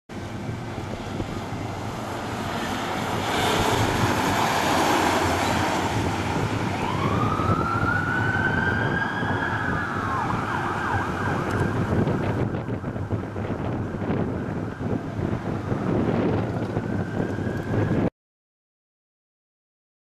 Akut Laege Kobenhavn
A Copenhagen medic car responding out of the central fire station. Filmed with a Sony HDR-CX505 //

by Dirk Steinhardt - Kobenhavn, Danmark - 08.2011